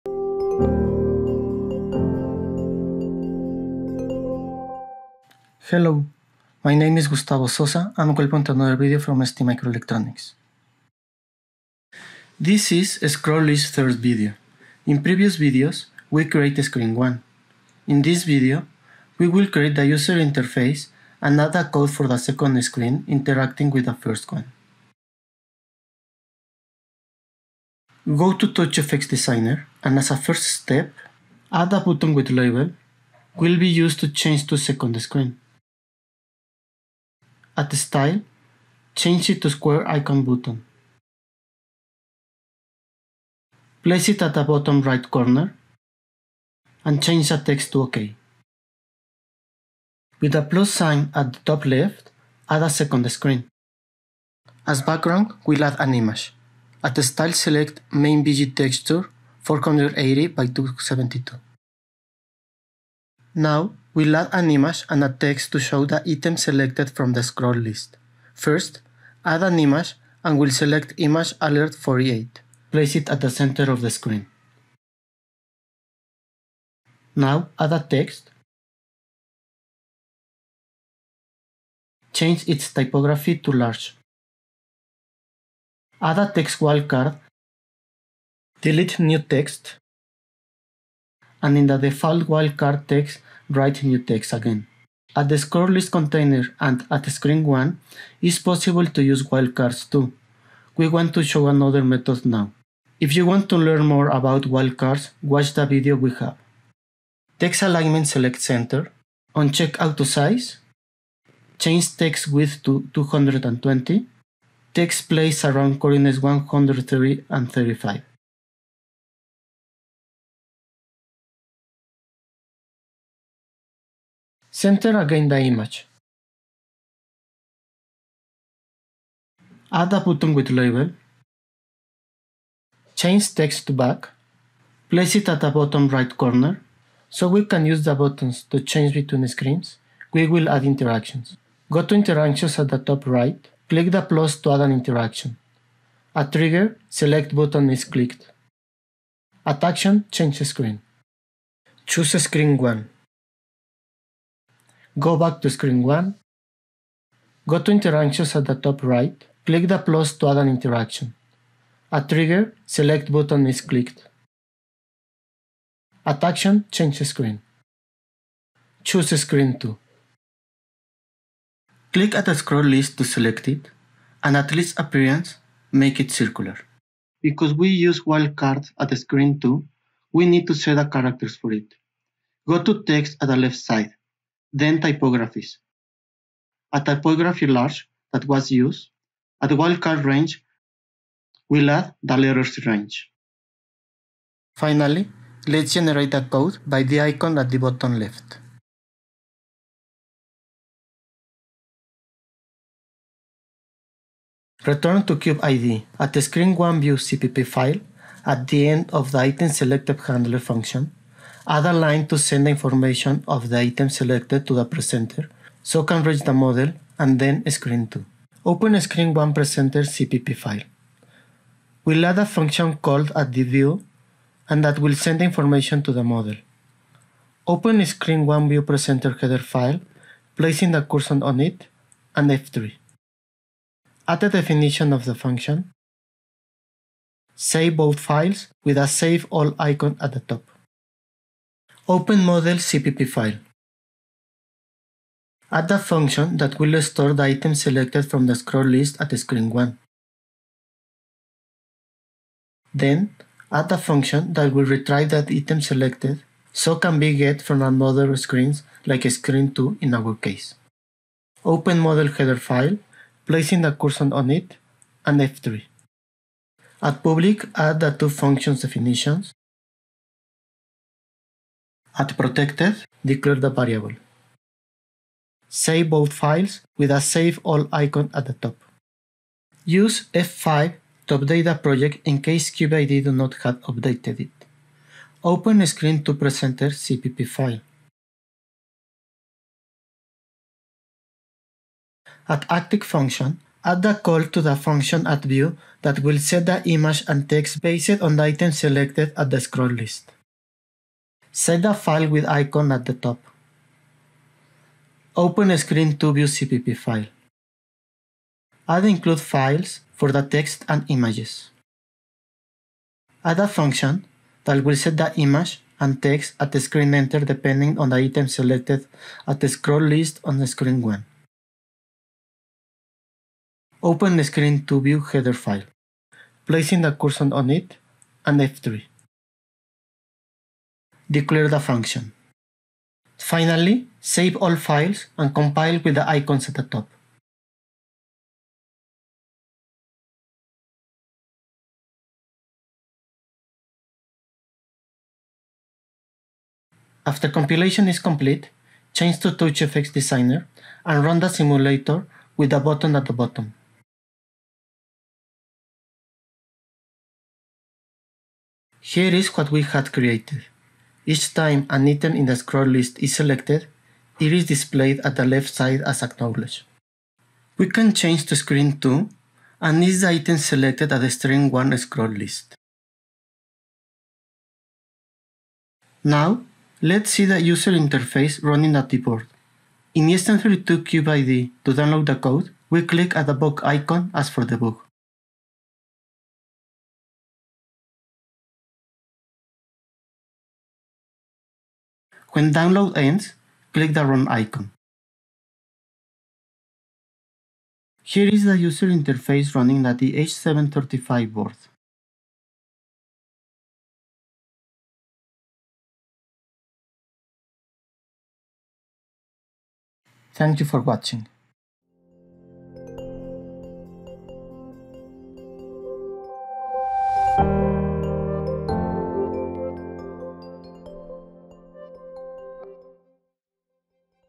STM32 Graphics: Getting Started with Scroll List Widget, Part 3
Learn how to use Scroll List Widget in TouchGFX. The concepts can be easily applied to other evaluation boards and custom projects. The assets (graphic images) and code used in this video are included below for easy inclusion in your own project.

Development boards used: STM32H735G-DK
Code snippets for additions and images available here (copy and paste):

00:00  Project information
00:27  Second screen addition
02:25  Adding interactions
02:55  Changing Scroll list to circular
03:03  Adding Wild Card characters range
03:22  Scroll List code for Screen 2 interaction
05:44  Demo 1 (Simulator)
06:36  Demo 2 (Board)